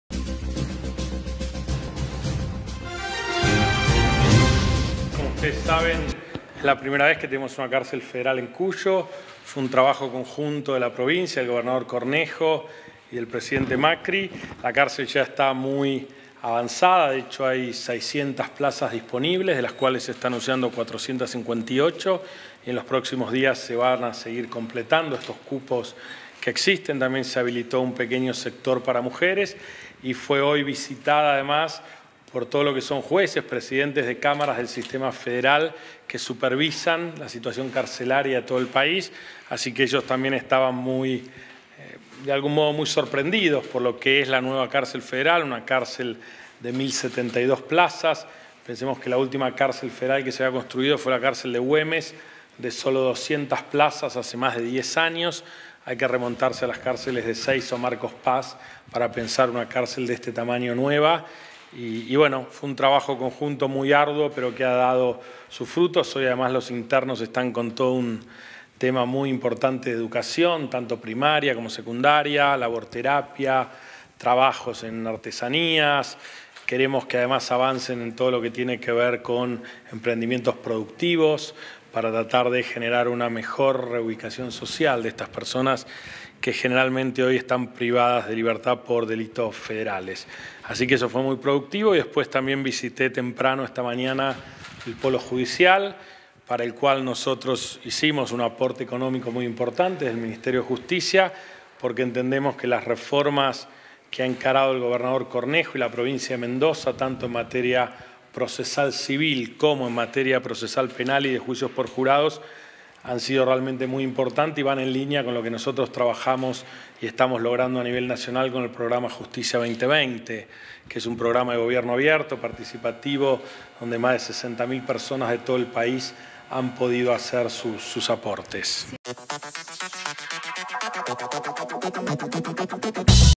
SISTEMA FEDERAL
El Ministro de Justicia de la Nación, Germán Garavano, vistió el complejo penitenciario federal 6, ubicado en el departamento de Luján de Cuyo. Junto a autoridades judiciales del ámbito federal recorrieron las obras, y los avances del futuro Polo Judicial.